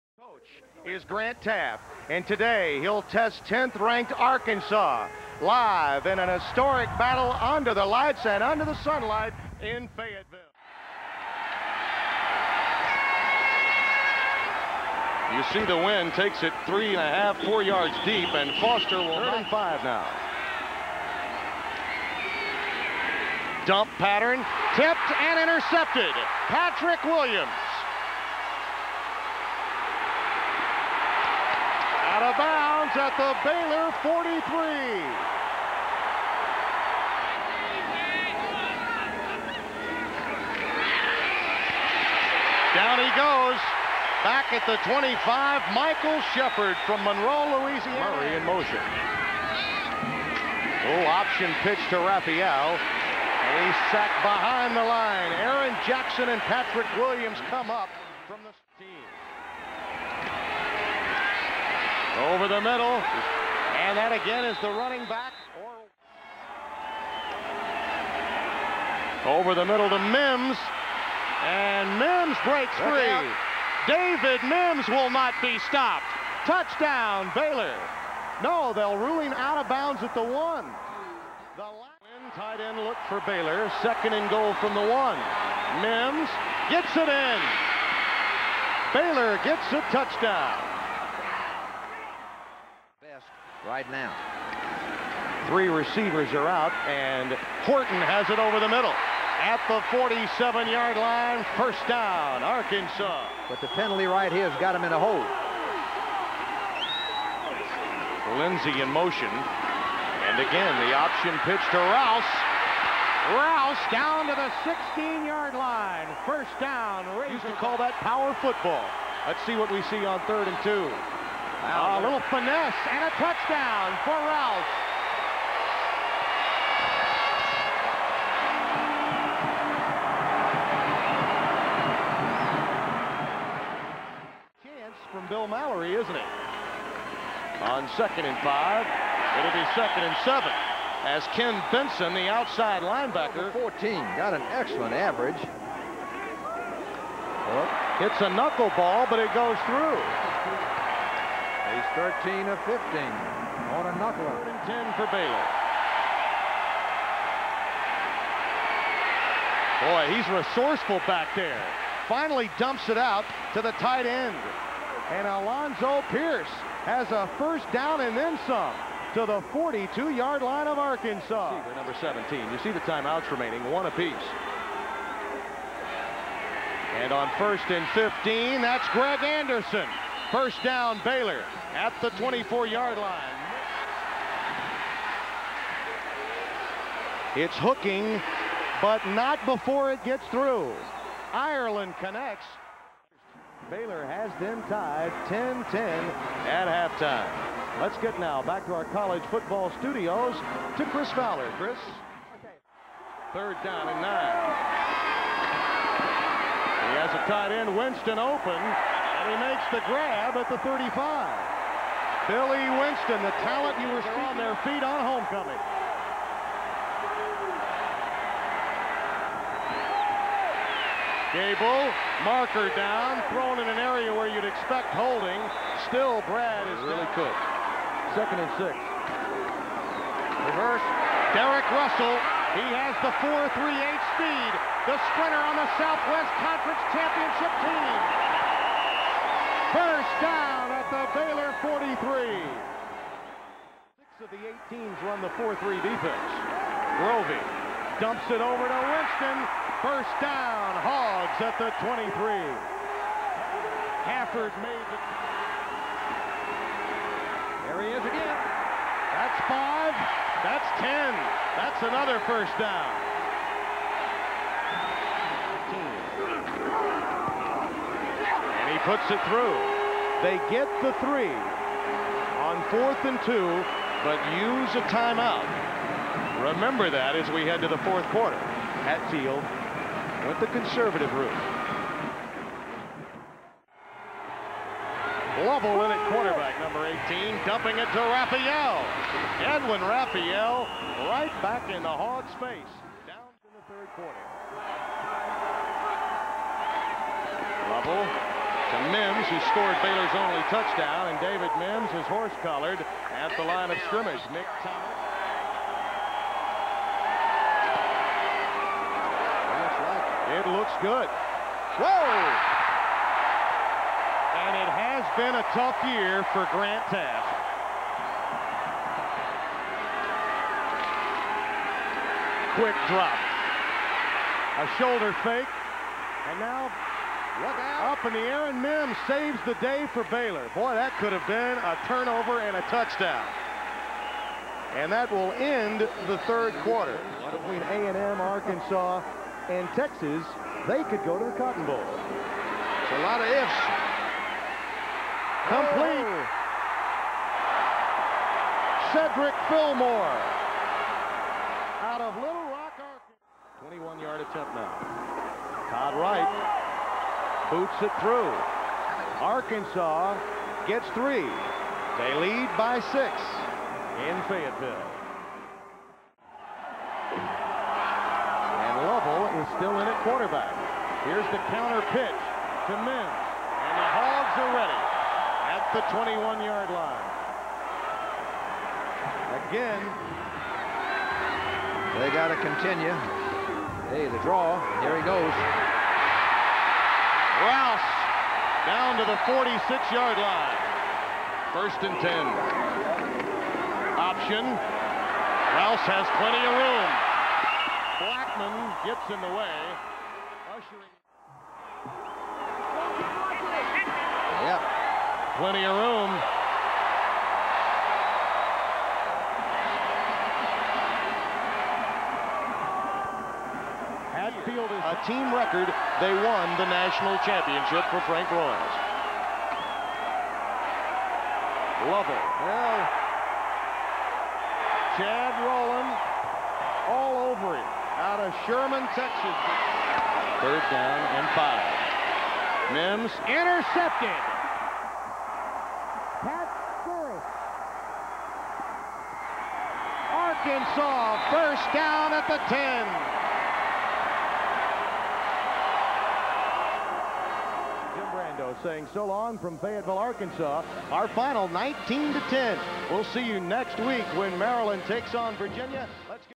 #10 Arkansas vs. Baylor 1989
Highlights of the #10 Razorbacks 19-10 victory over the Bears in 1989.